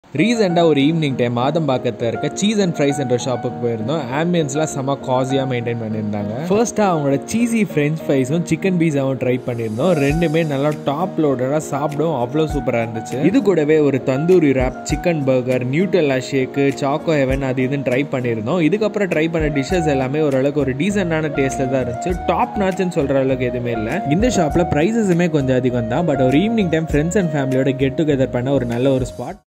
🧀 Cheese N Freeze 🍨 Adambakkam 😋 Food Review
Cheese N Freeze in Adambakkam is restaurant with well maintained ambience, We tried their cheesy french fries which was loaded with Mayo and cheese. Then Their Special Chicken Pizza which was fully loaded as well, Both These Dishes were good and a must try if you visit here.

The other dishes we tried were chicken burger, Nutella Shake, Choco Heave, Tandoori Wrap Etc,. These Dishes were fine not the best. A Good Place for a Get together with family and friends with decent tasting food. Prices are but Higher Too!

Cheese N Freeze
2/142, Medavakkam Main Rd, Jeevan Nagar, Adambakkam, Chennai, Tamil Nadu 600088